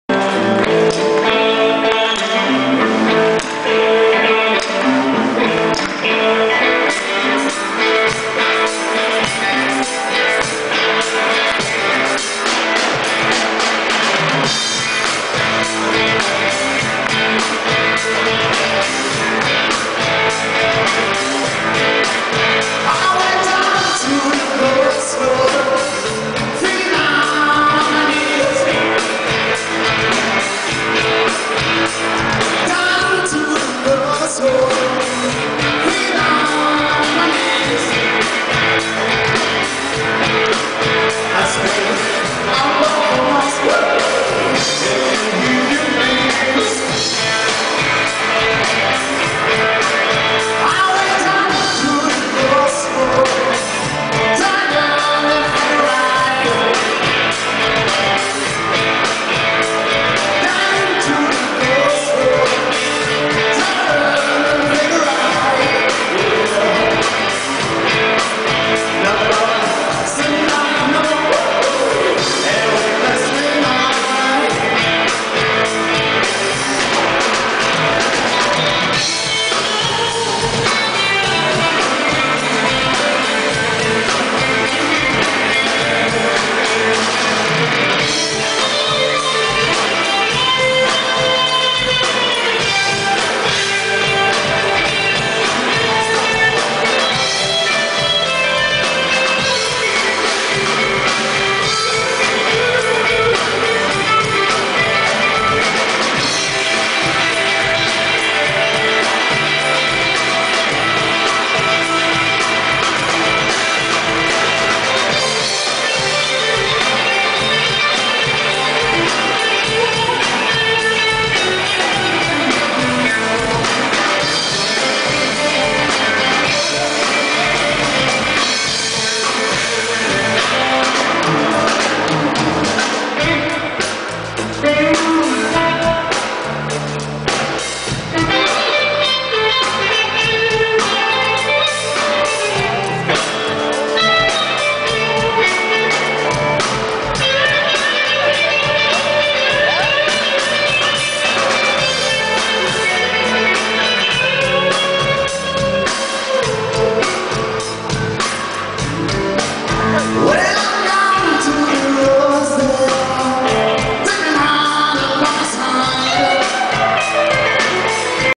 JEAN-CLAUDE RAPIN ET PAUL CREA / LIVE GUITARES EN SCENE 2009
Mister Jean-Claude Rapin (grande pointure de la six cordes) et Paul Crea (le régional tout aussi doué de l'étape), s'ouvrent l'appétit en nous servant un plat préparé avec les ingrédients du crossroad ! La démarche est fort louable et surtout cette envolée à deux grattes riche en saveurs. De plus on sent bien que la bouture est plantée dans le plaisir de jouer. Deux musiciens à qui on ne l'a fait pas et qui rayonnent véritablement en arpentant le devant de la scène. (Guitares en scène 2009 - St Julien en Genevois).